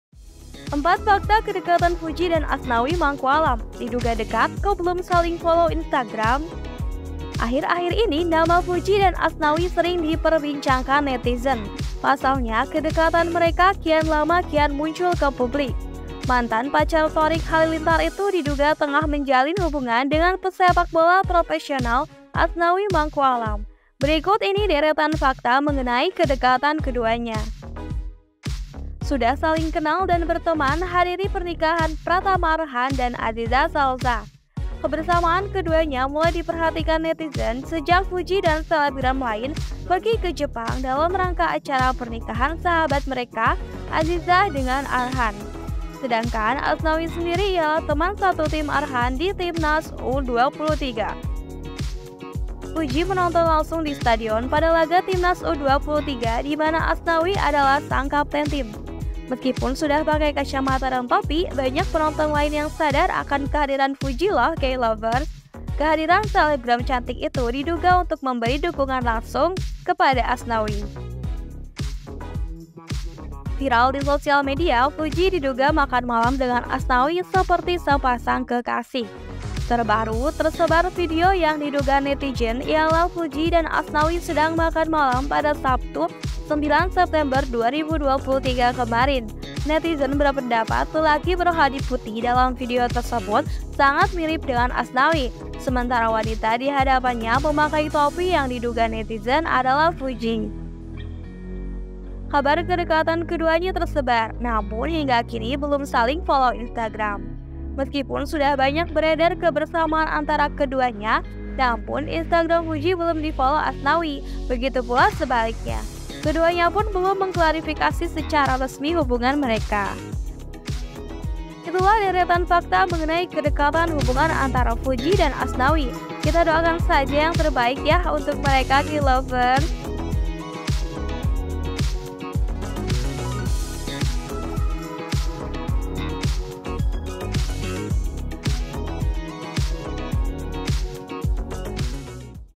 4 Fakta Kedekatan Fuji & Asnawi Mangkualam, Diduga Dekat Kok Belum Saling Follow Instagram?
Kisah asmara selebritis yang sedang ramai dibicarakan muncul dari adik ipar mendiang Vanessa Angel, Fujianti Utami atau yang akrab dipanggil Fuji. Setelah putus dari Thariq Halilintar, Fuji diduga sedang menjalin hubungan dengan Asnawi Mangkualam, pesepakbola profesional. Mau tau gak bagaimana perkembangan hubungan Fuji dengan atlet bernomor punggung 14 itu? Tonton disini ya!